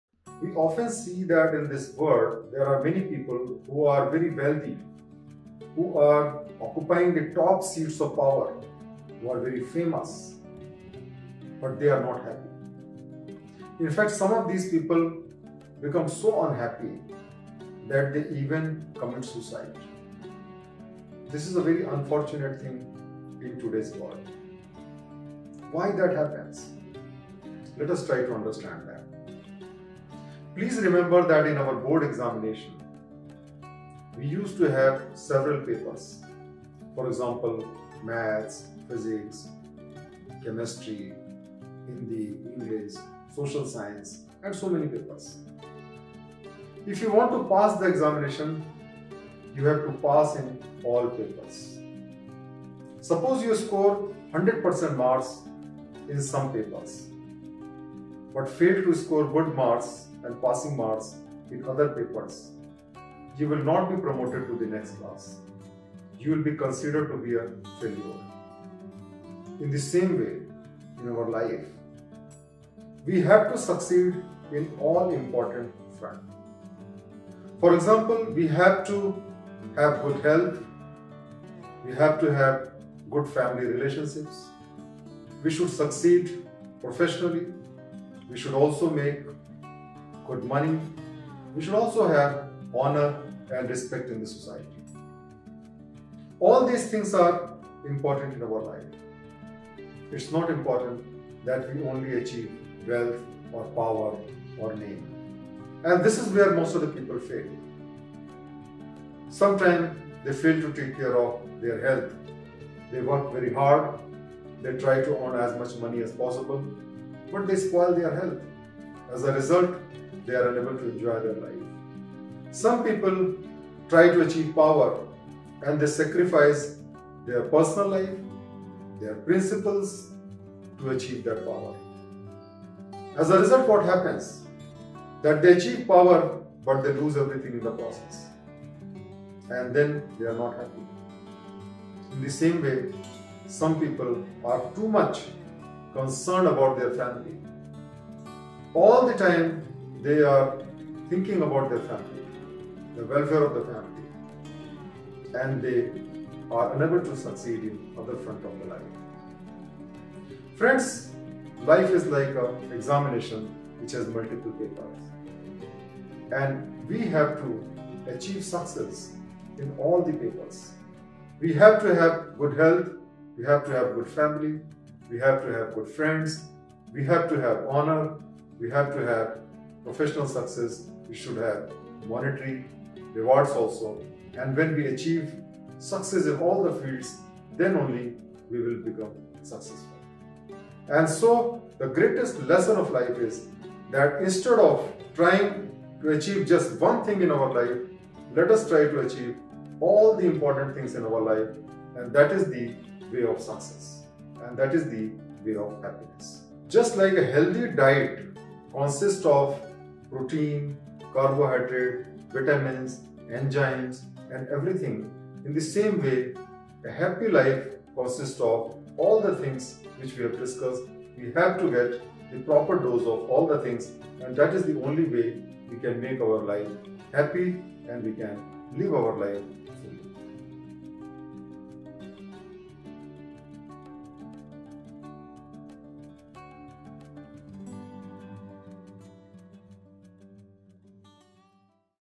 How to achieve sustainable success and happiness | Dr. Awdhesh Singh
How to achieve sustainable success and happiness
Must Watch this video to know...
Like the page to watch new life-skill videos in Hindi and English regularly.